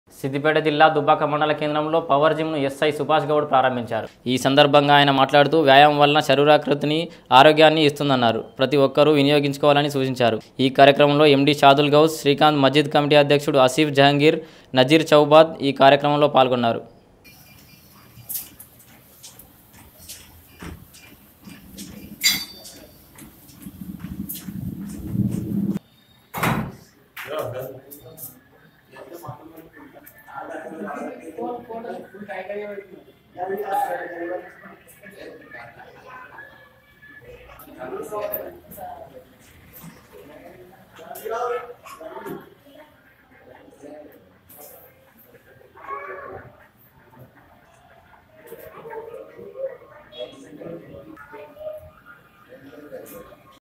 TS24 News పవర్ జిమ్ వ్యాయమ శాల ను ప్రారంభించిన దుబ్బాక SI సుభాష్ గౌడ్
జిమ్ వ్యాయమ శాల ను ప్రారంభించిన దుబ్బాక SI సుభాష్ గౌడ్